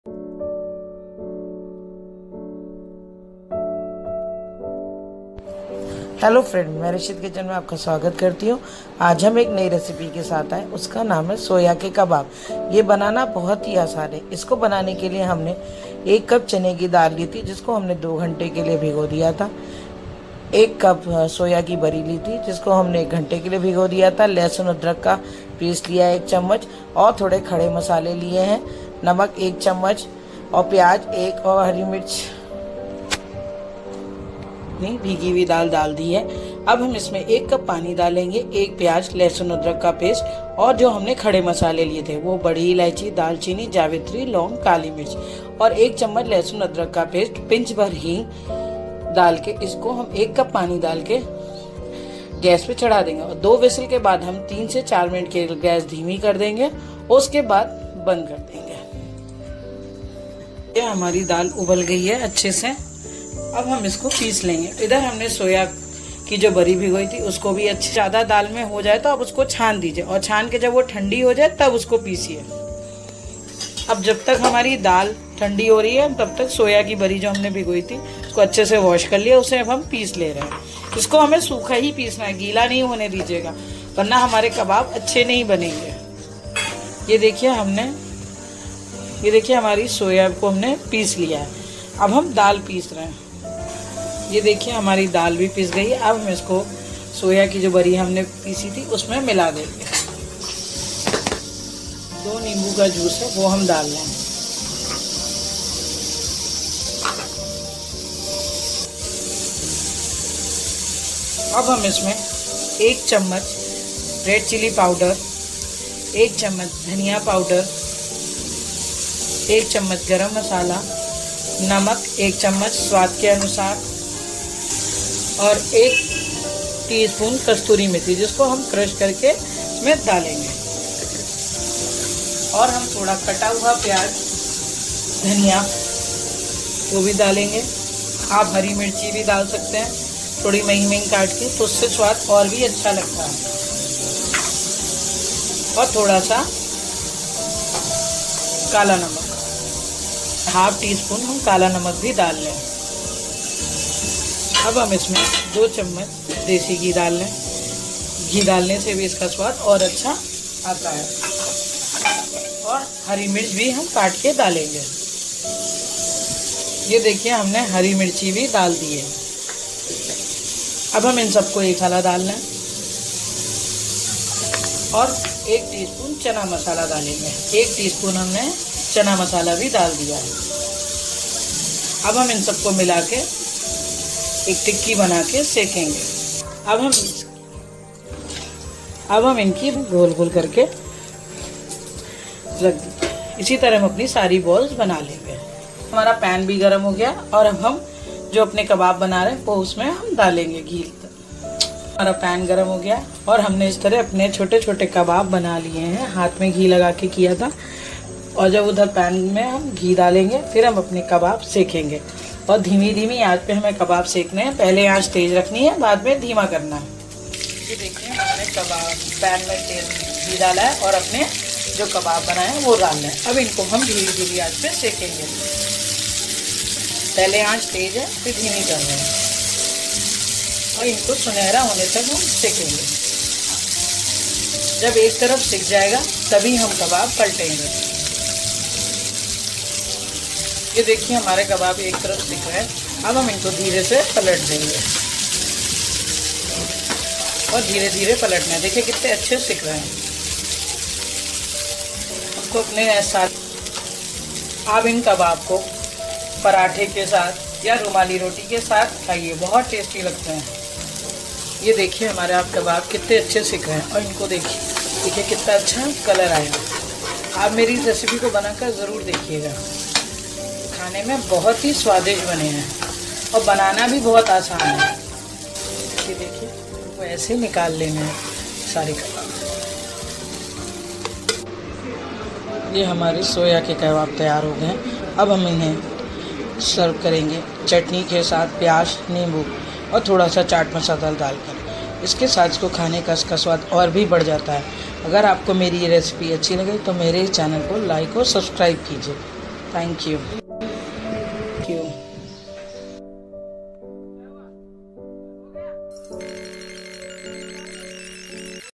soya kabab # delicious # food lovers # super easy recipe
1 cup chana dal
1 cup soya
2 onions chopped
2 to 4 green chillies chopped
1 teaspoon ginger garlic paste
2 teaspoon lemon juice
salt as per your taste
5 to 6 kali mirch
3 to 4 cloves
1 black cardamom
2 green cardamom
half teaspoon jeera
1 teaspoon red chilli powder
1 teaspoon coriander powder
1 teaspoon garam masala powder
1 teaspoon chana masala
1/4 cup chopped coriander leaves
2 teaspoon desi ghee